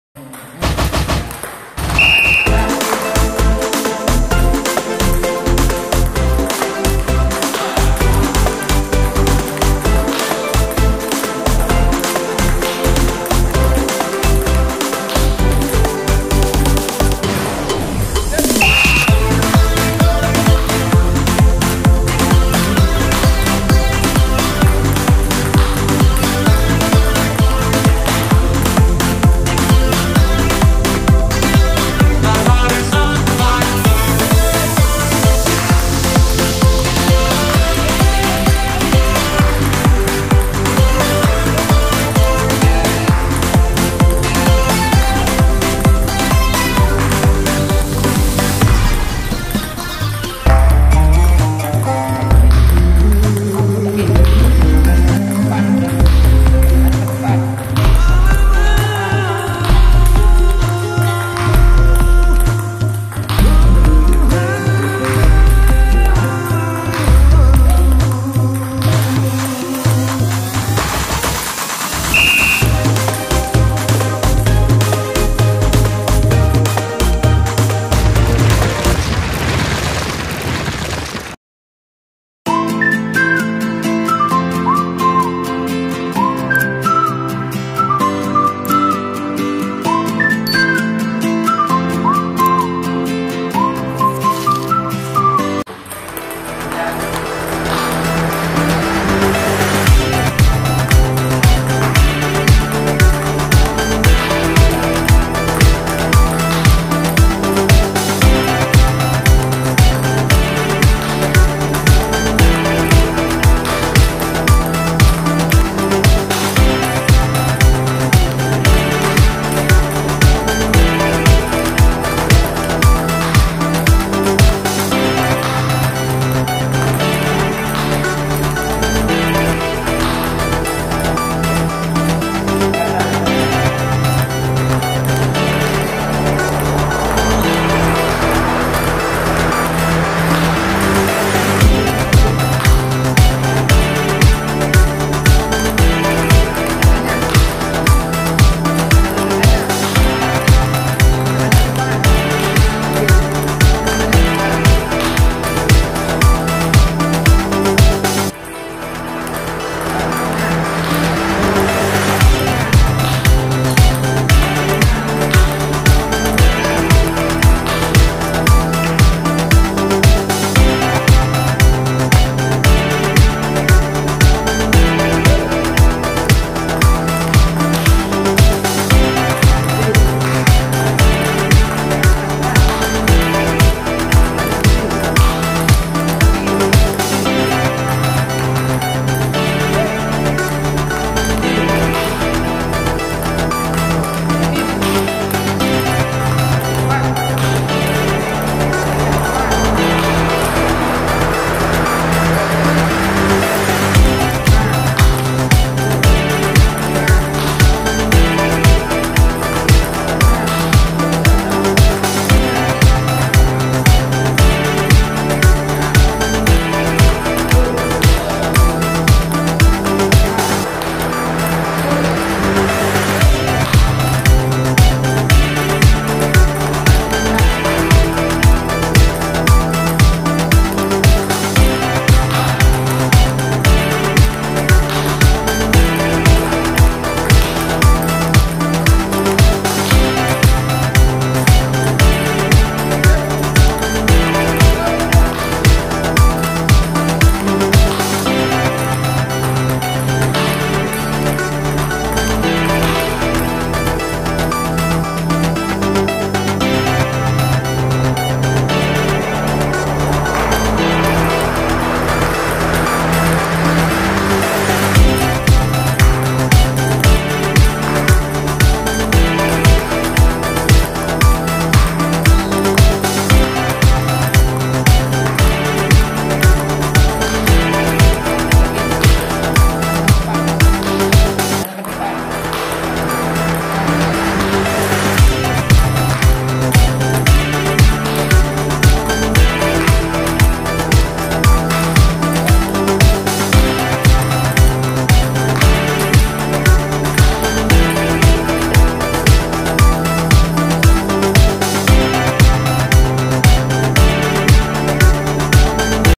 Bintik Racun..!! Latihan Forehand dgn Binser Dr.Neubauer Killer
Latihan Bolban di PTM.Kejora Jakarta

Saya Ucapkan Terimakasih kepada semua yg mendukung Channel ini Semoga selalu Sehat dan dilimpahkan Rizqi yg Barokah..
 Aamiin ..🤲